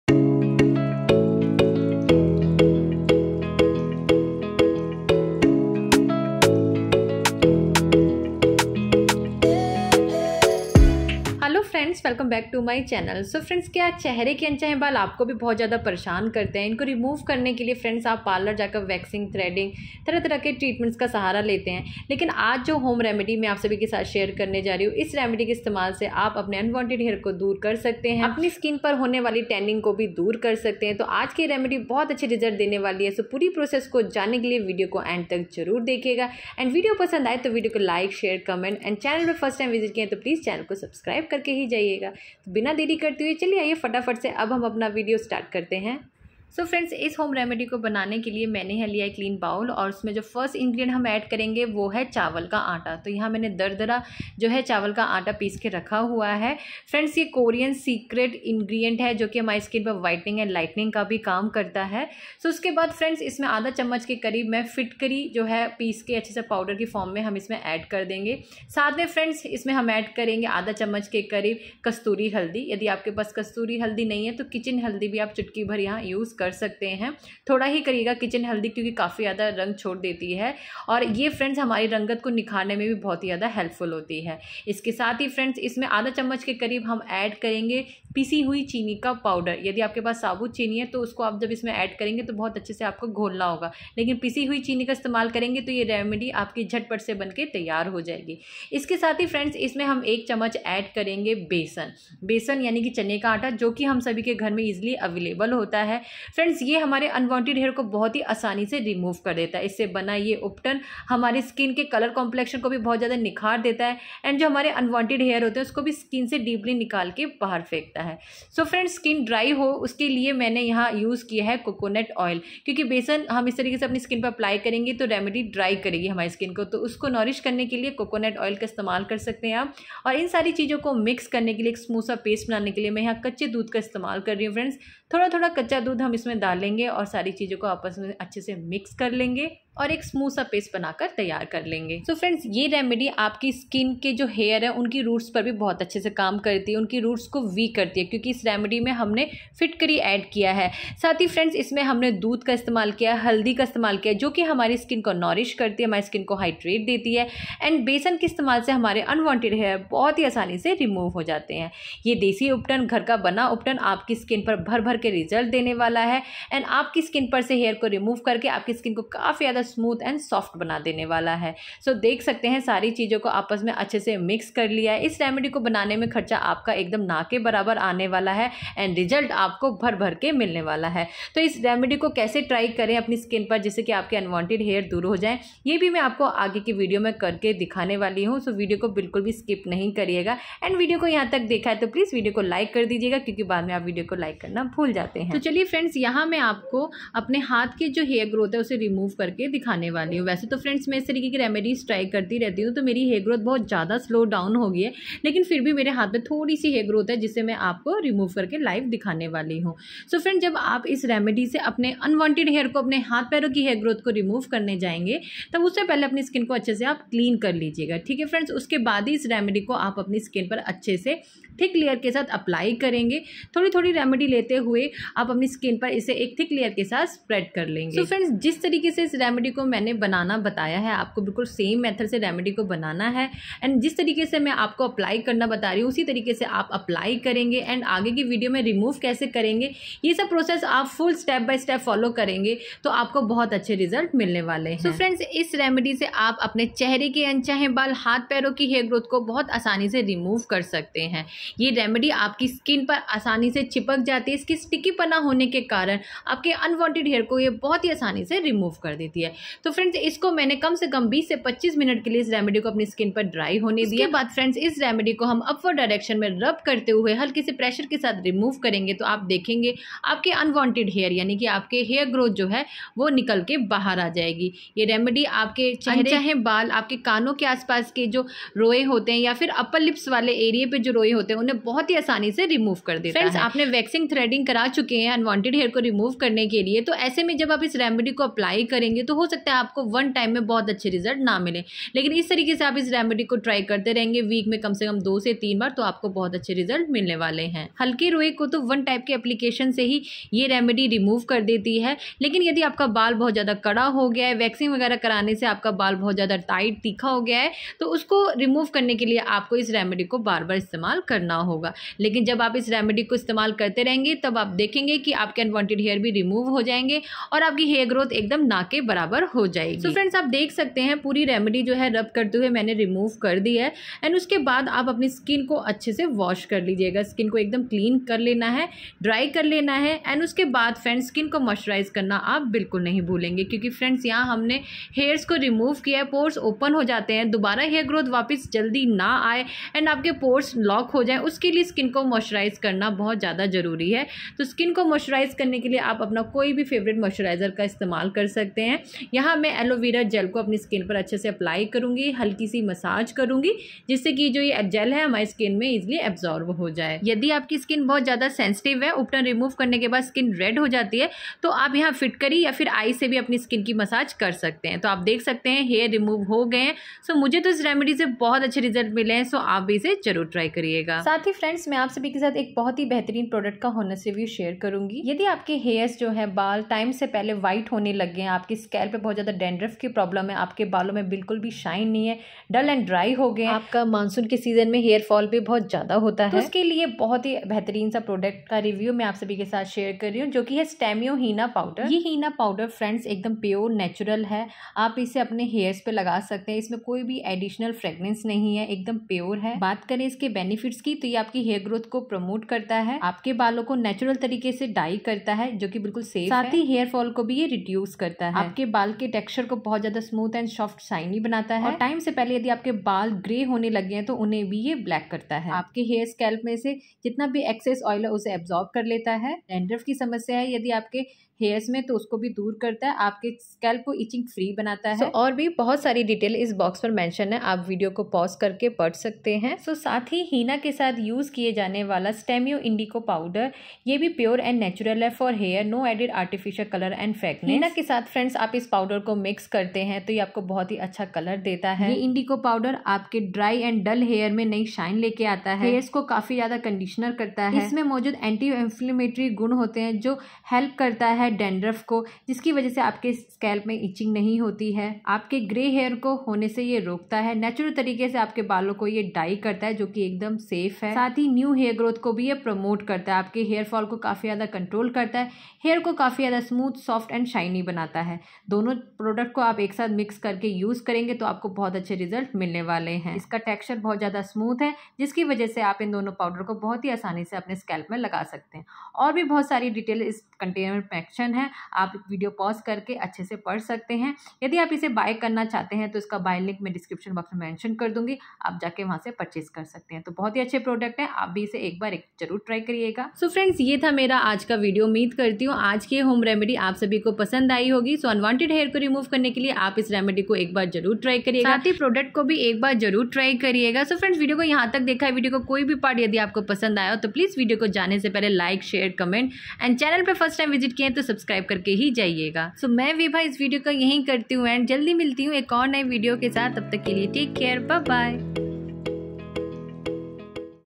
बेसन और शहद में एक चीज मिलाकर🤔चेहरे के मोटे बाल भी निकले दो मिनट में😱100% || Remove facial hair
hello everyone
kaise hai aap
आज की वीडियो में मैं आपको बहुत ही कमाल का घरेलू रेमेडी शेयर कर रही हूं जो लगाते ही तुरंत आपके फेस के मोटे तीखे अनचाहे बाल को साफ कर देगा यकीन नहीं तो लगा कर देख लो बस एक बार

👉buy link-

Stamio Indigo powder
Stamio Henna powder
Stamio Jamun seed powder